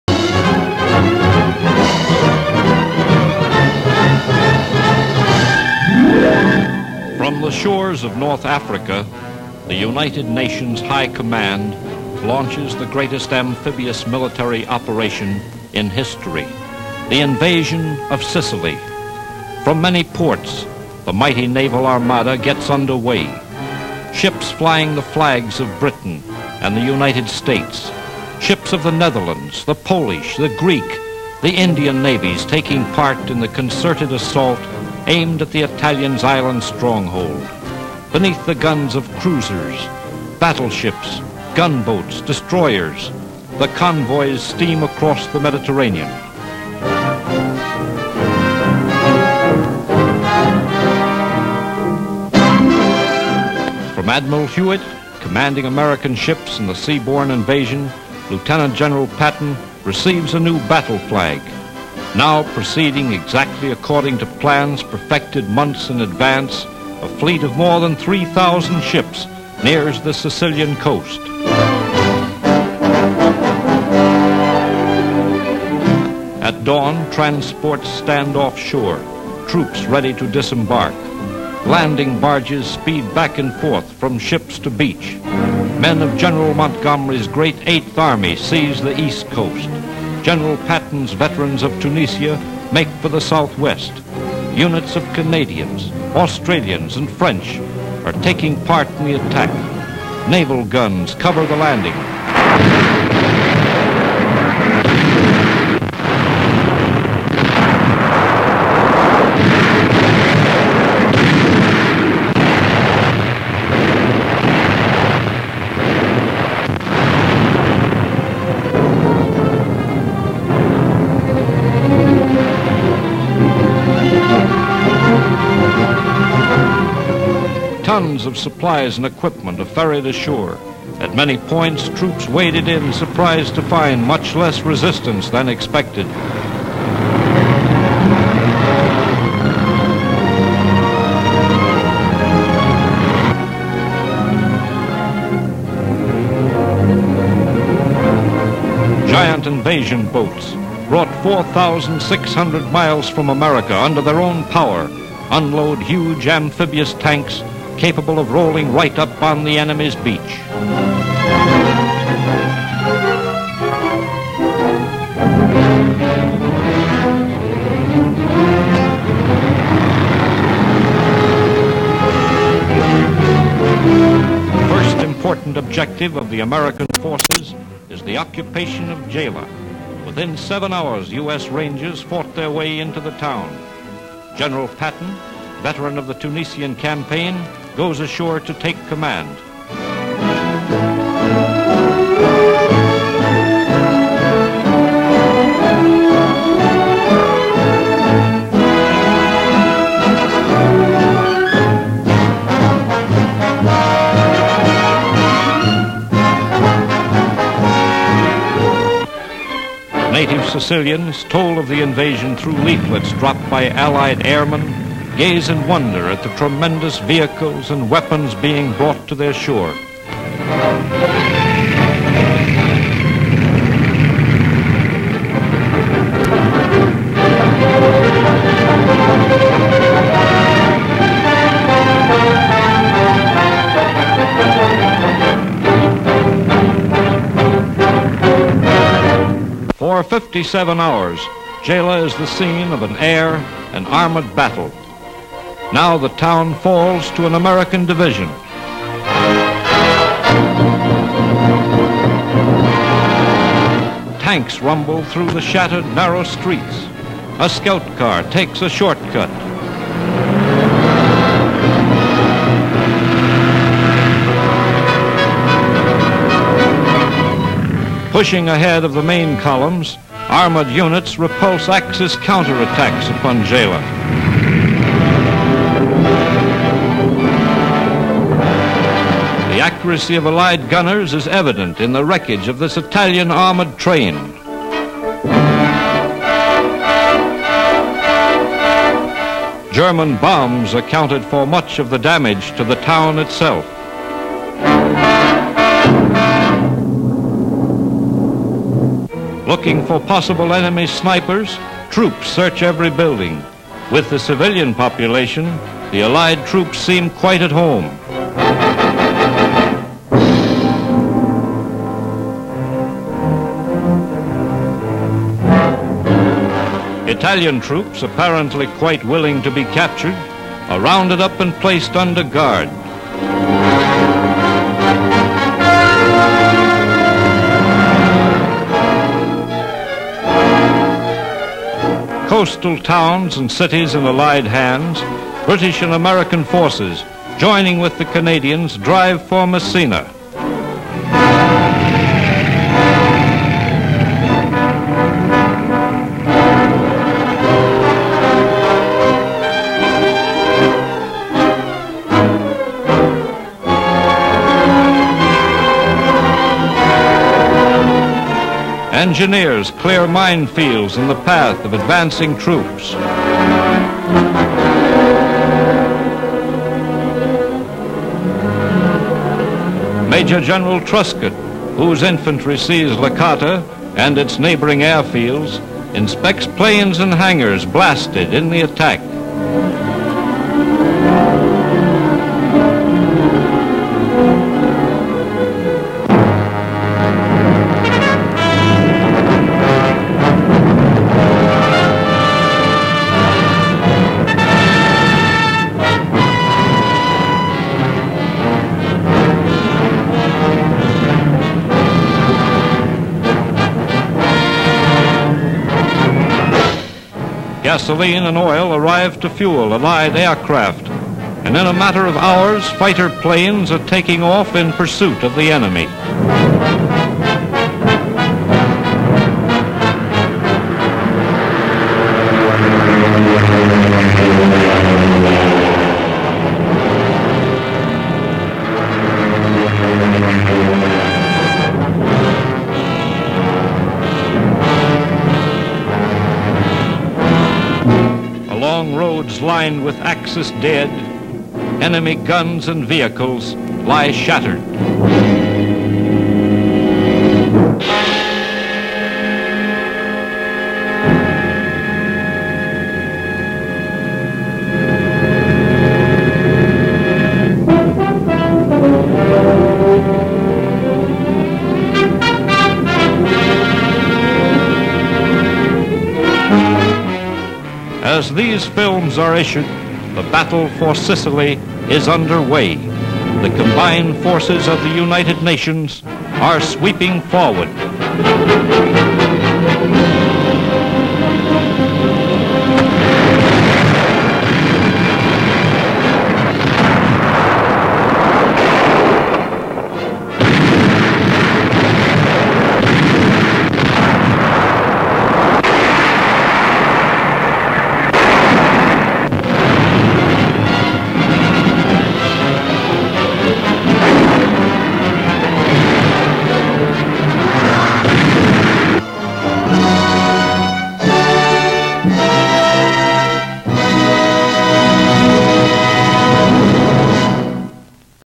Historic Archival Stock Footage WWII - The Invasion of Sicily!
Military Newsreels 1943 Issue 21
Invasion of Sicily. An invasion fleet of over 3.000 ships forms on the shores of North Africa and crosses the Mediterranean Sea for the invasion of Sicily. General George S. Patton receives a new battle flag. Shows the amphibious assault on Gela, Sicily. Land mines are cleared and troops advance. Shows General Lucian Truscott.